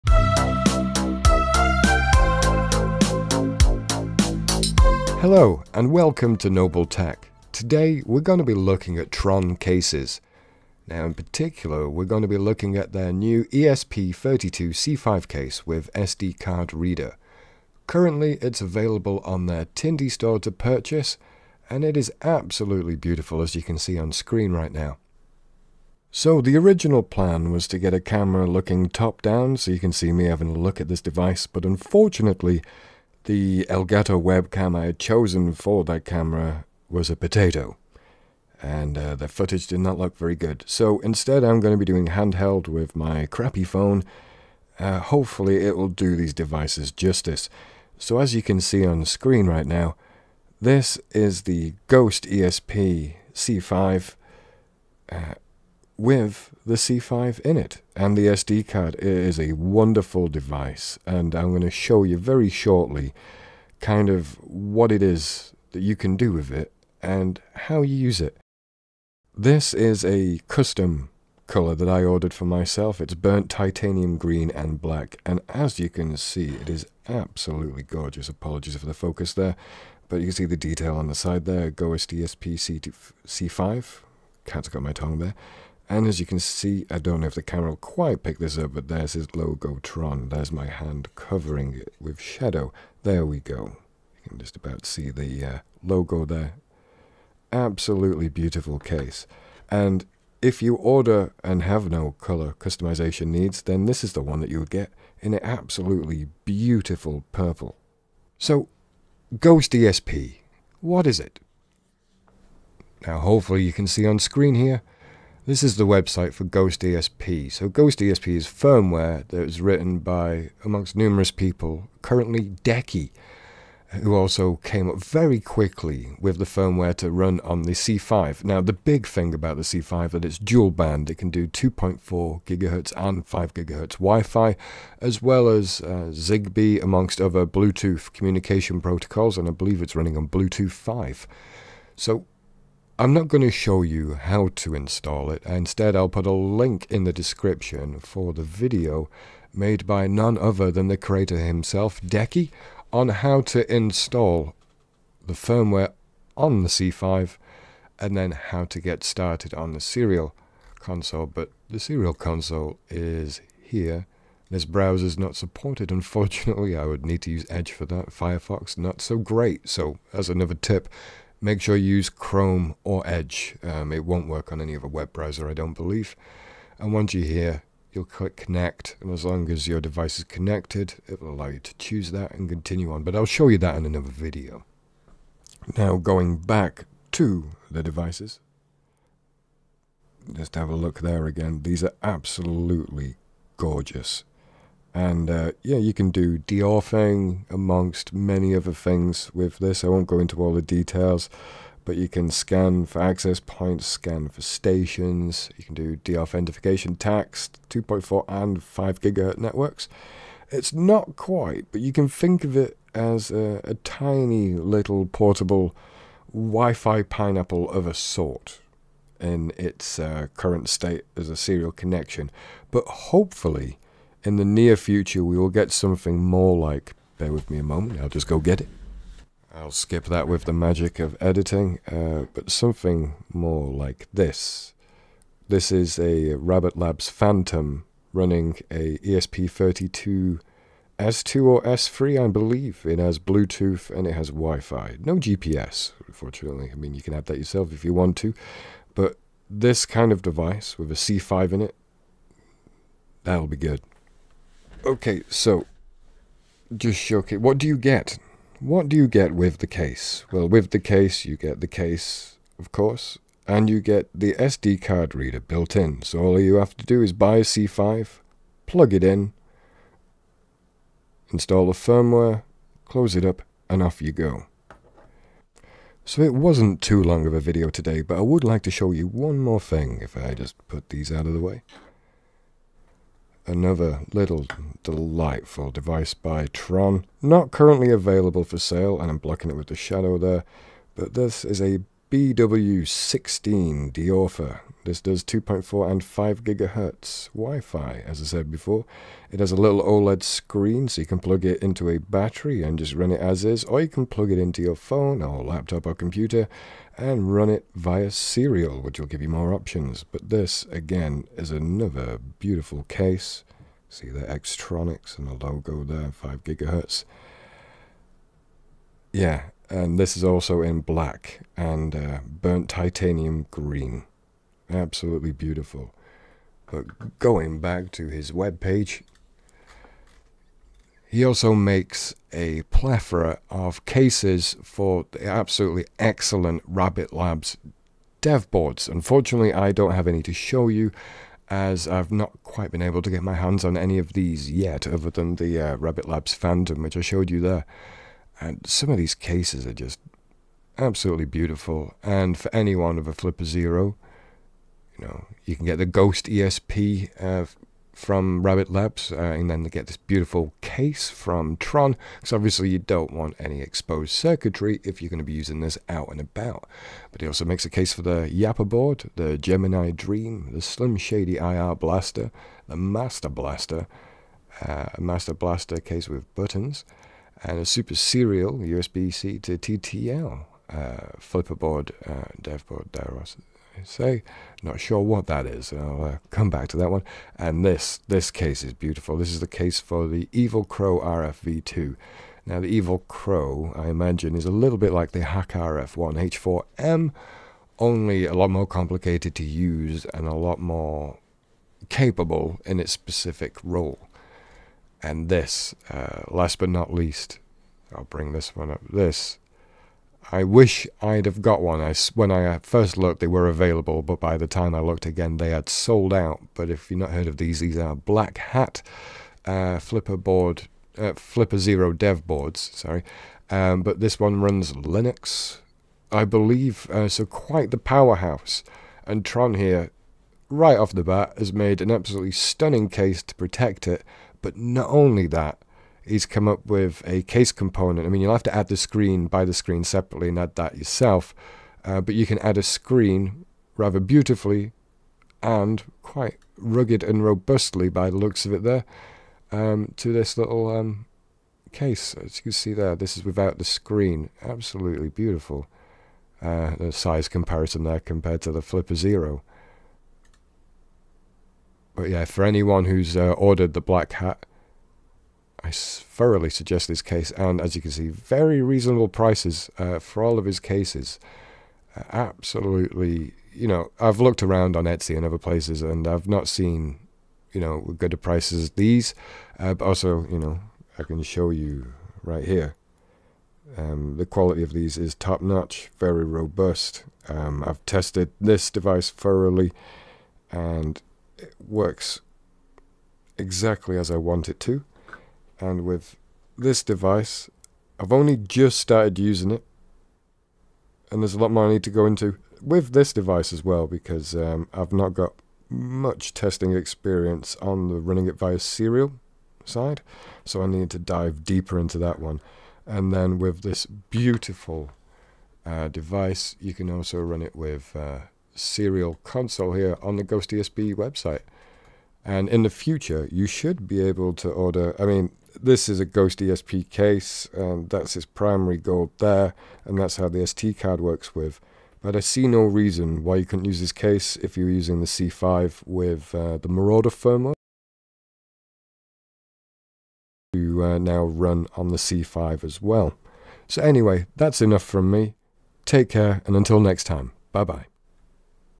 GhostESP ESP32 C5 Case with SD Card Reader by Tron
Welcome to Nobul Tech,

In today's video I review an amazing case for the ESP32 C5 by Tron.

With the C5 you can deauth dual band wifi and more.

I acciently screen recorded in 1080p so apologies for the potatoe resolution